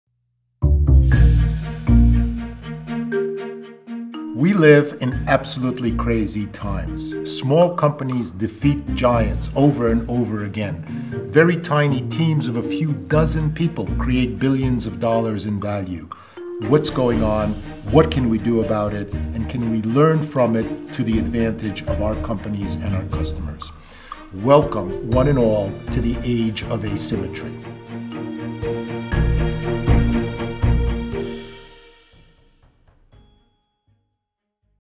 Як перемагати на глобальних ринках? – Адріан Сливоцький
«Малі компанії перемагають гігантів ринку знову і знову. Крихітні команди з кількох десятків людей створюють вартість у мільярди доларів. Що відбувається? Що нам з цим робити? І чи здатні ми навчитися з цього задля переваги наших компаній та наших клієнтів? Ласкаво просимо усіх до епохи асиметрії», – каже провідний бізнес-футуролог світу Адріан Сливоцький. Проте він вже знає, як створити вашу власну модель, що дозволить перемагати на глобальних ринках.